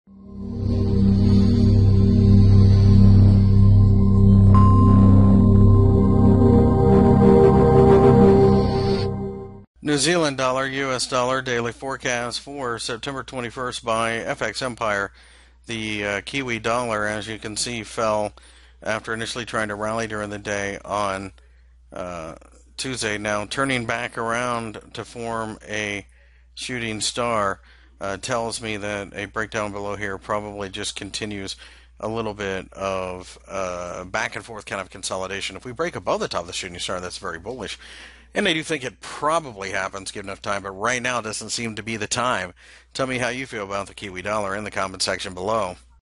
NZD/USD Technical Analysis for September 21 2016 by FXEmpire.com
- September 21 2016 currency daily technical analysis for the NZD/USD pair. Forex Technical Analysis Forex Fundamental Analysis and Forex Brokers on FX Empire